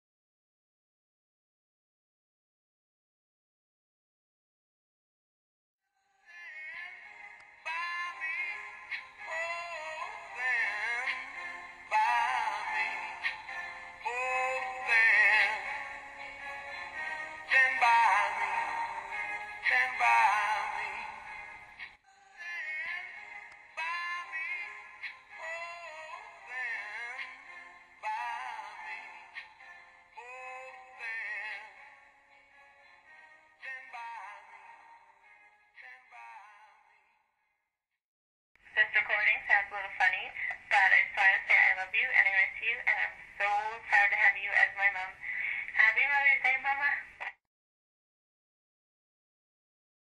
A Beautiful Mothers Day card
My daughter Amber sent me this card a few years ago when she was away with Katimavik.
This is the actual music from the card; it starts to play when the card is opened.
She left a special voice message recorded within the card.
The short video at the end was an accidental recording I made of her with my iPhone.

On this first Mother's Day of her passing, I want to express how extremely fortunate I was to have had my daughter for 21 years. I would not trade a moment that I shared with Amber!